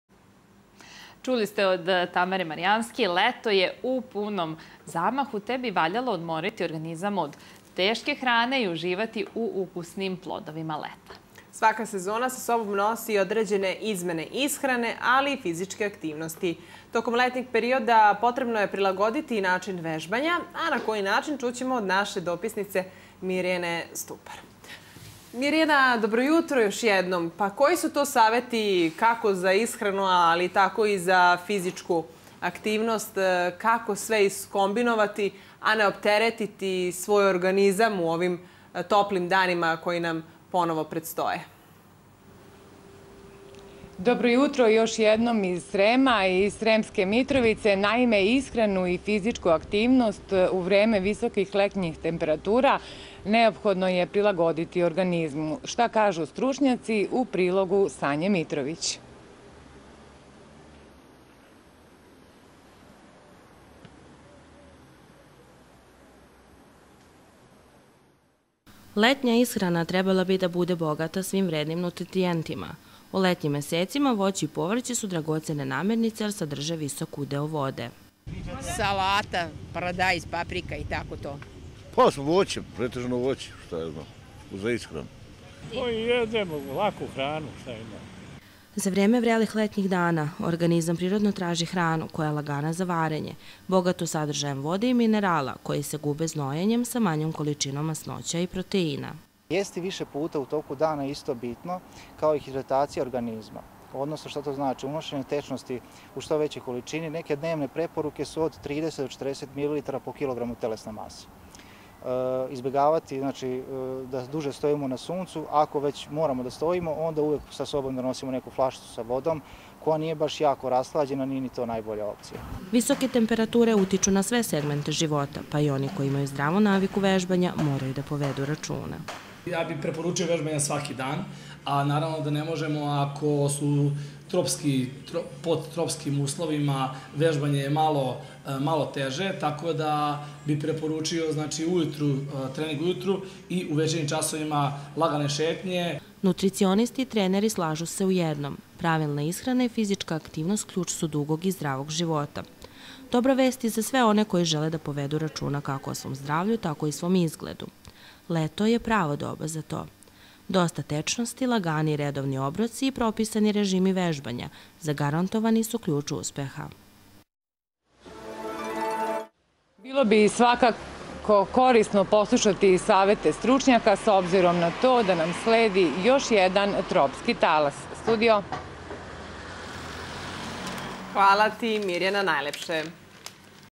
Ishrana i način vežbanja tokom leta
Leto je u punom zamahu te bi valjalo odmoriti organizam od teške hrane i uživati u ukusnim plodovima leta. Svaka sezona sa sobom nosi i određene izmene ishrane, ali i fizičke aktivnosti. Tokom letnjeg perioda potrebno je prilagoditi i način vežbanja, koji su saveti kako za ishranu, ali tako i za vežbanje?